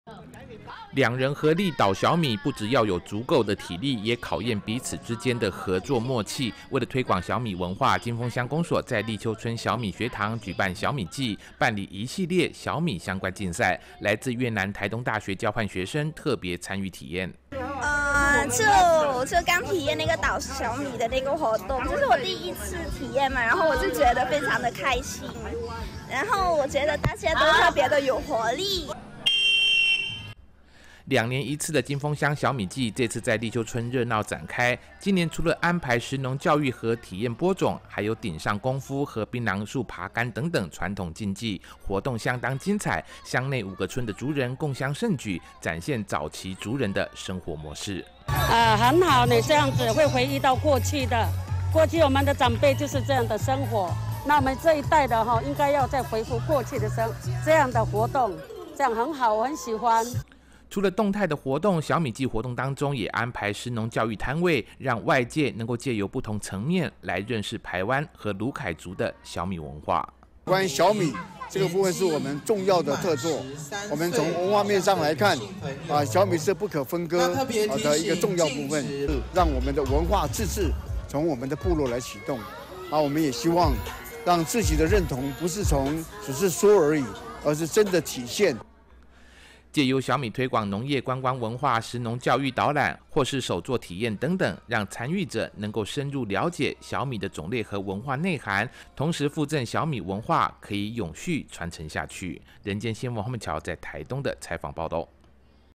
20240814展現原民部落食農文化 台東金峰鄉舉辦小米祭
兩年一度的金峰鄉小米祭，日前盛大熱鬧舉辦，除了有食農教育及體驗活動，為展現傳統原住民的生活模式，也安排了許多的競技比賽，藉由趣味活動，讓參加民眾更加深對部落傳統文化的認識。